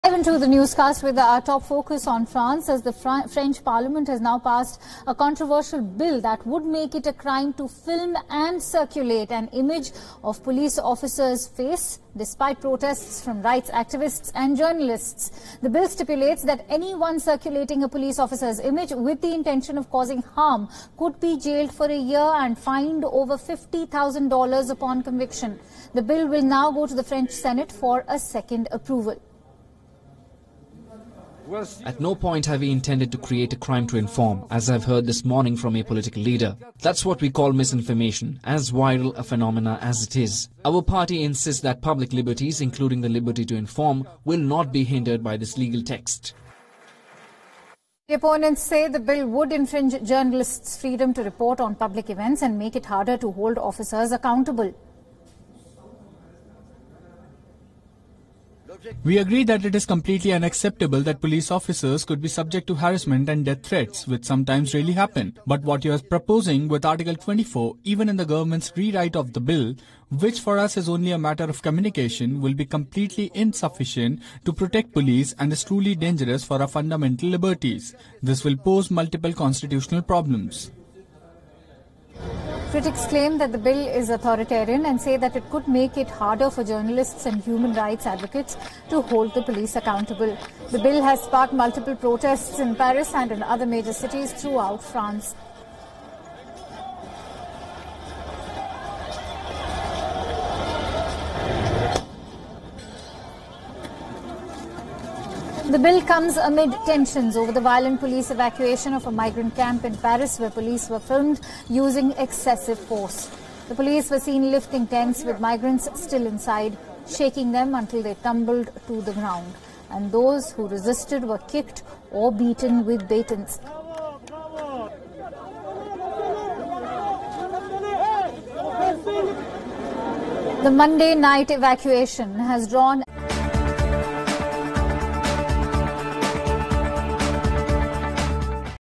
Your Story: France approves controversial "Global Security" bill, law curbs identification of Police
French Parliament has now passed a controversial "Global Security" bill that would make it a crime to film & circulate an image of Police officer's face despite protests from rights activists and journalists.